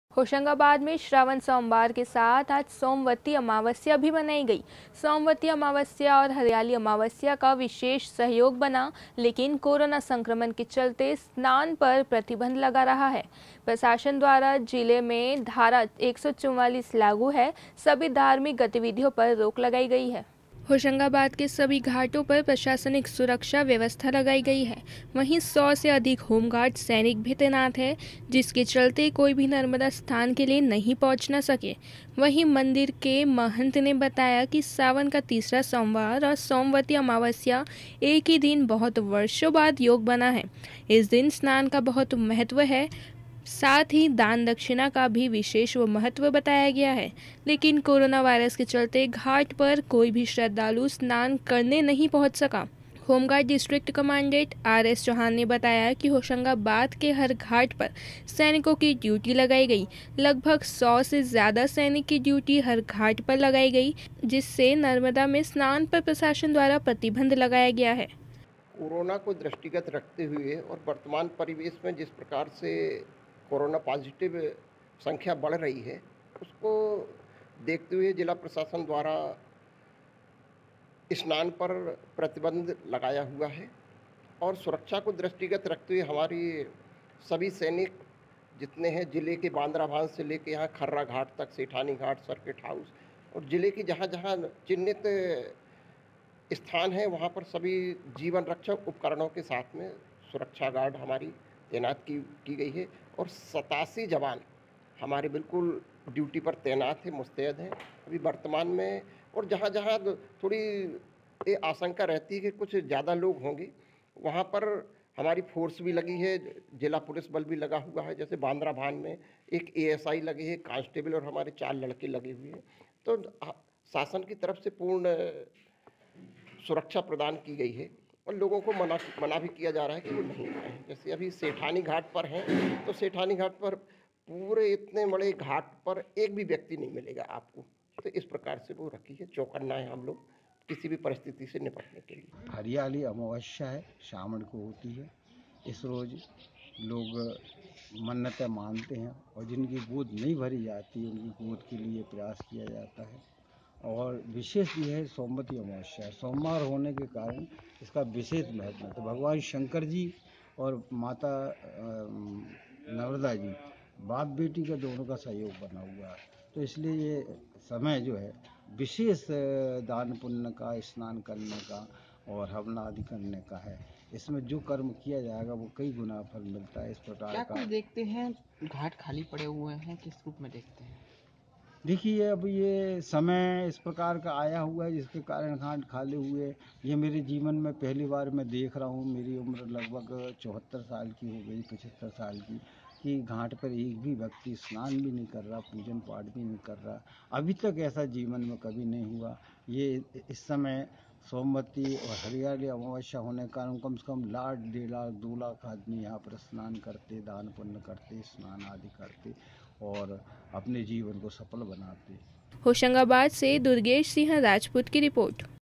HOSHANGABAAD सोमवती अमावस्या
होशंगाबाद में श्रावण सोमवार के साथ आज सोमवती अमावस्या मनाई गई । सोमवती अमावस्या और हरियाली अमावस्या का विशेष सहयोग बना लेकिन कोरोना संक्रमण के चलते स्नान पर प्रतिबंध लगा रखा है । प्रशासन द्वारा जिले भर में धारा 144 लागू है। सभी धार्मिक गतिविधियों पर रोक लगाई गई है।